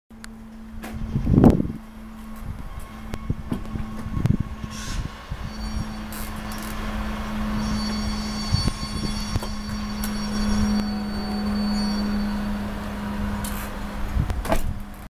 Auto Ventura - Range Rover Sport SDV6 HSE Dynamic - achterklep
Deze Range Rover Sport SDV6 HSE Dynamic is o.a. voorzien van een elektrisch bedienbare achterklep. Bij het ontgrendelen ziet u bovendien de elektrisch uitklapbare treeplanken in werking.
Verkrijgbaar bij Auto Ventura in Goes